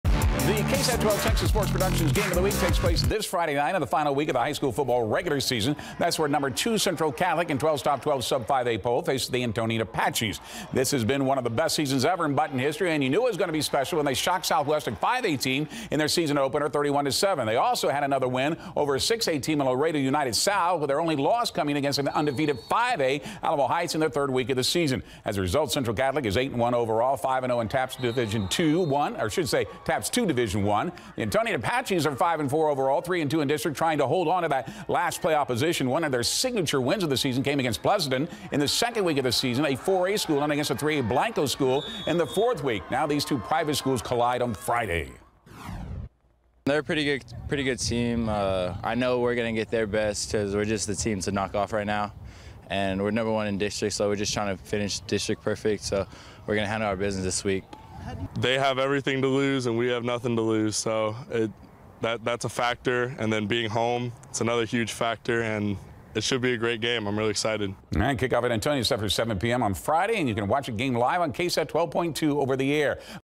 BGC TSP Game of the Week Preview: Central Catholic vs. Antonian
Week 11’s matchup between the Central Catholic Buttons and Antonian Apaches is Friday night’s Game of the Week, part of KSAT 12's partnership with Big Game Coverage and Texas Sports Productions to broadcast live high school football games every week on air and online via multiple streaming platforms.